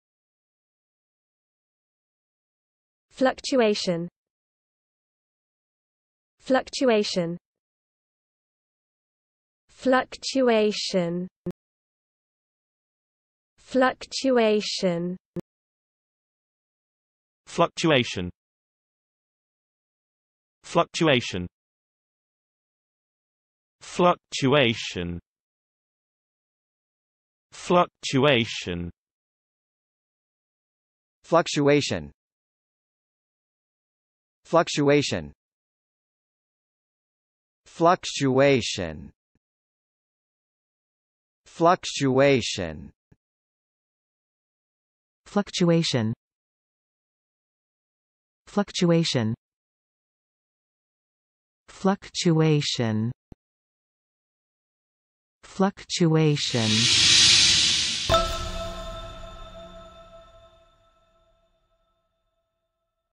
How to Pronounce "Fluctuation"
Need help learning English?

Welcome to the guide on how to pronounce "Fluctuation".

Oops! We're missing a definition for "Fluctuation", can you help us?! Let's chat about Fluctuation in the comments.

Do you fancy requesting a hard word?